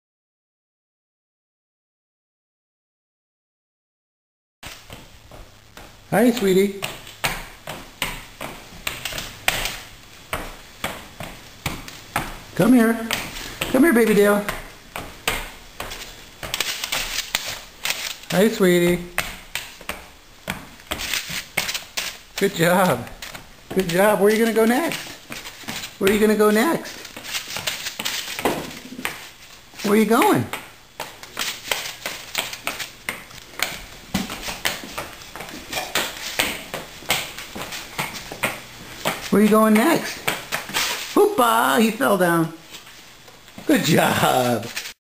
Baby Dale walking around the house
Baby Dale walking around the house only one week after taking his first steps.  Amazing.